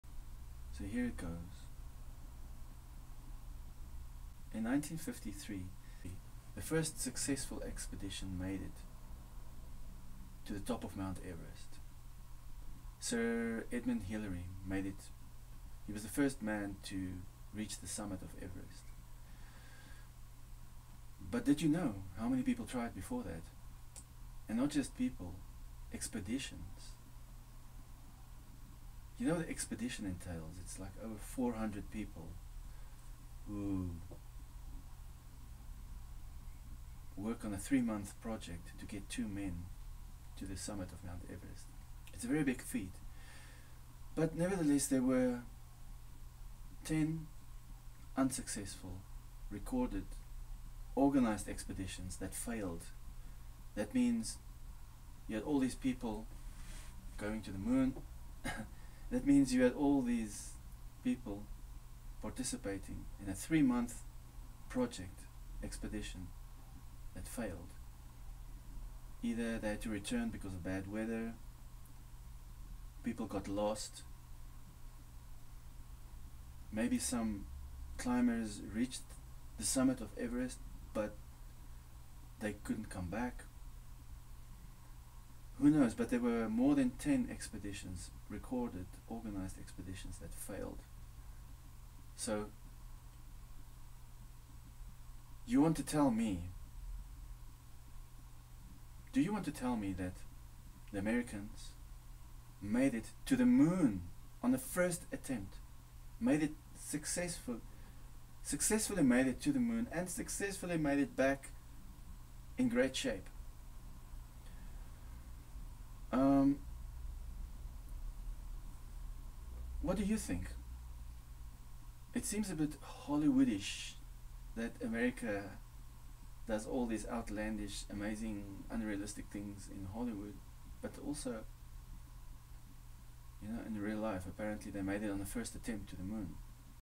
Moon landing - did it really happen?
Adding another element to the Moon Landing conspiracy theory that I feel is the most common sense reason to be REALLY skeptical